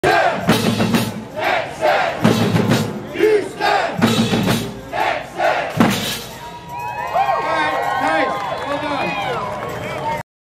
Texans 2012 Kickoff Party -- Pep Band #4
Houston Texans pep band at the team's kick off party at the Kemah Boardwalk in Kemah, Texas.  September 2, 2012.